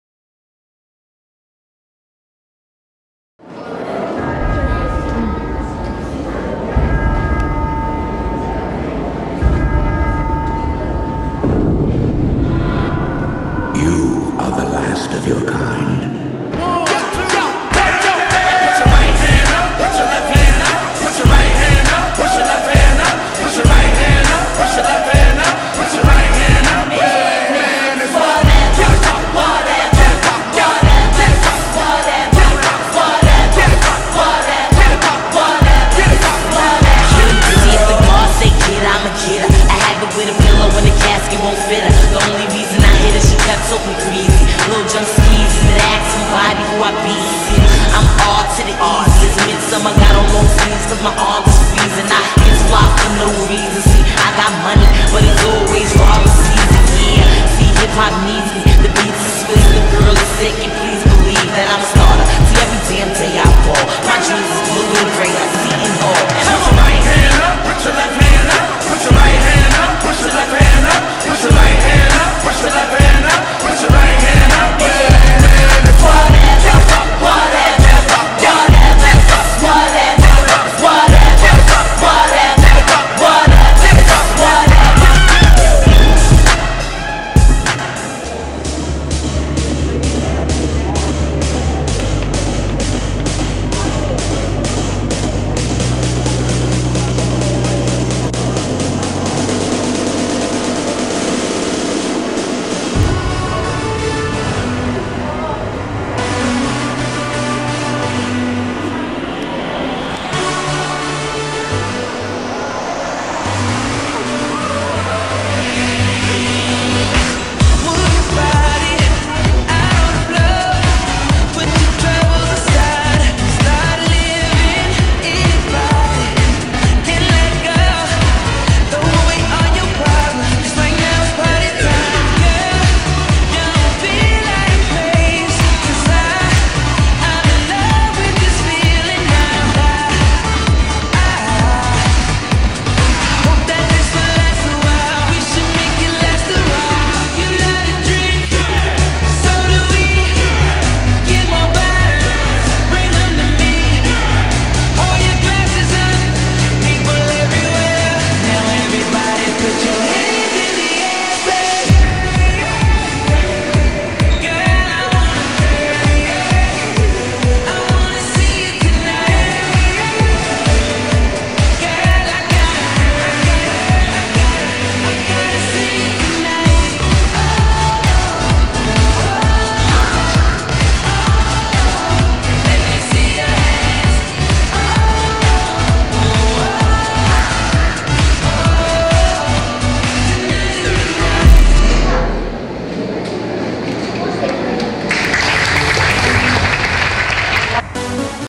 Recognition and Culmination Day- Dance GIFT Program
*sorry kung madaming mistakes, we tried our very best tho' .

Dancers:
Sheldone Dacalos
Arielle Ozamis
Angela Biliran
Gabriel Biliran
Claire Bretana